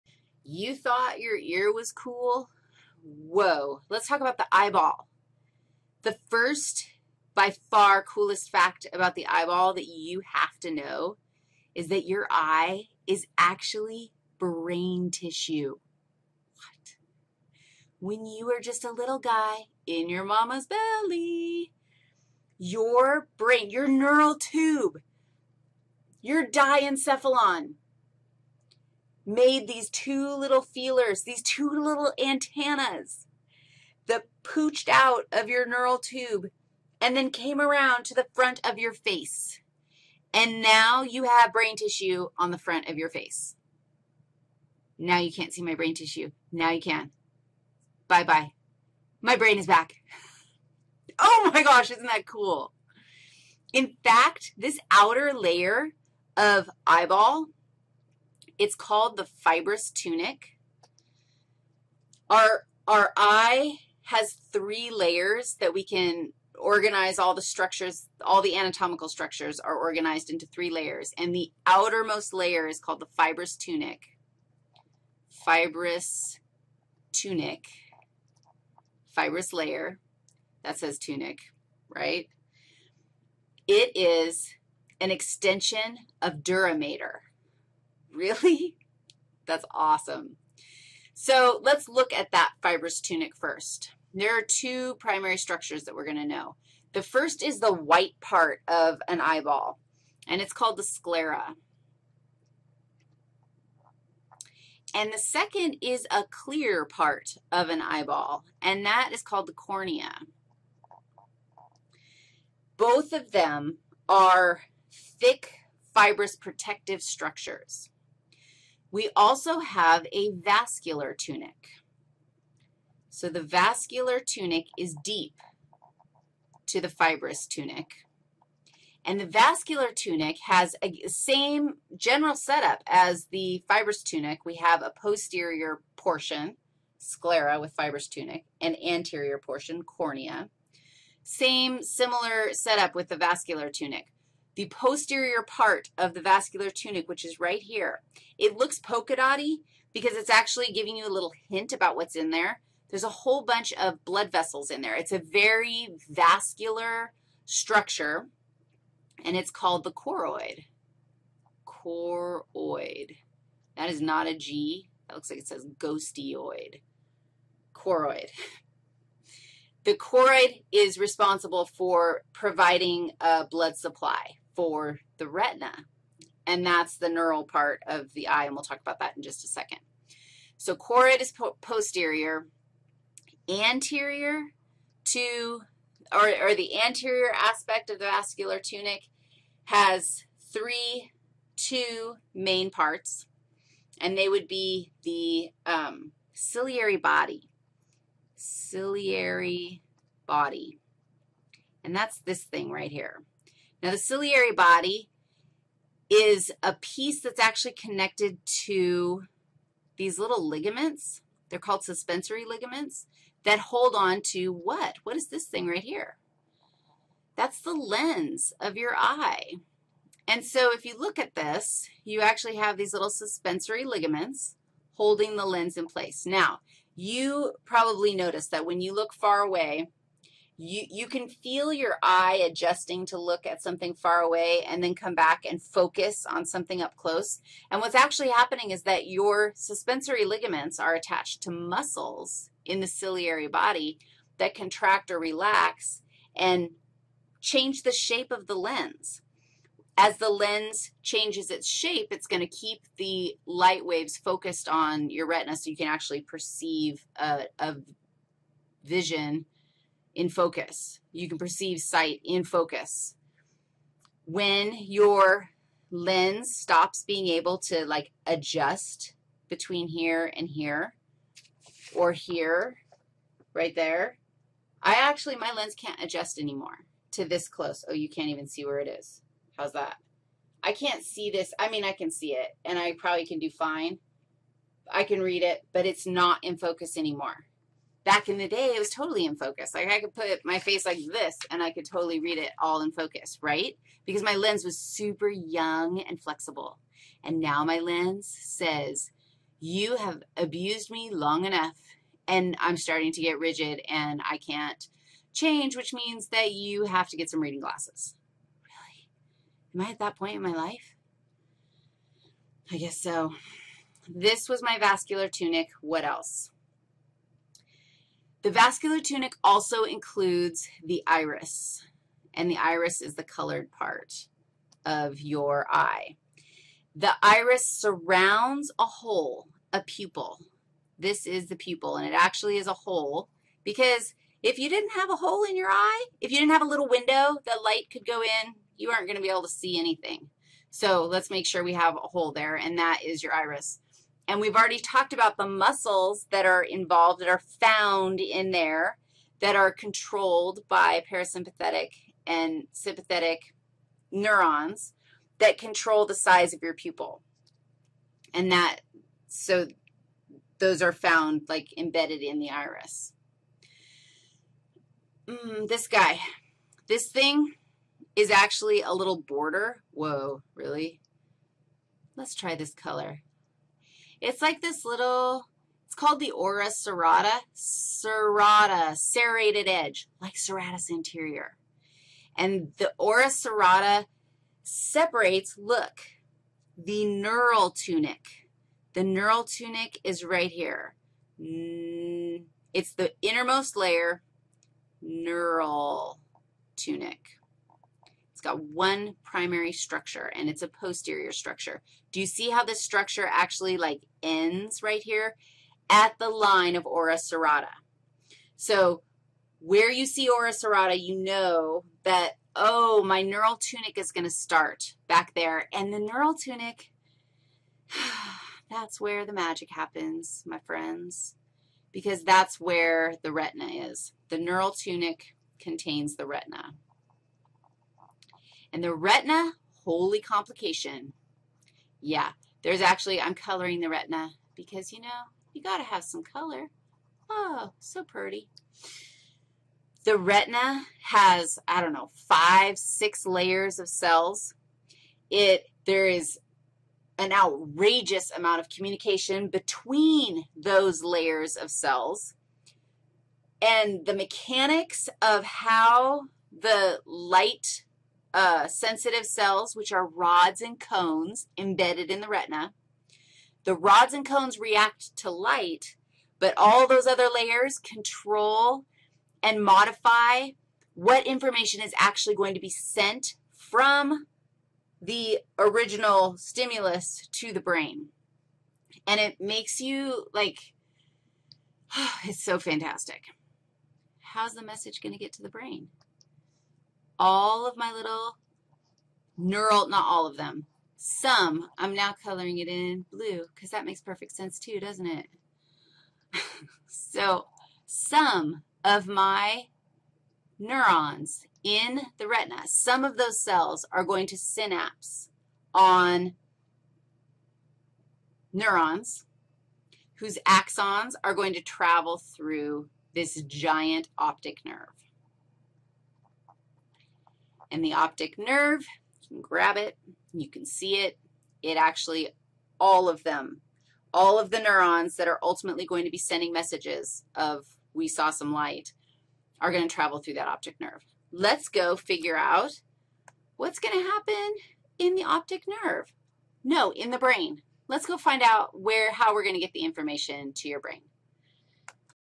Special Senses 8- Visual anatomy
Part 8 in a 9 part lecture on Special Senses in a flipped Human Anatomy course taught by Wendy Riggs. CC-BY. Watch the whole lecture (all 9 videos) by going to the PLAYLIST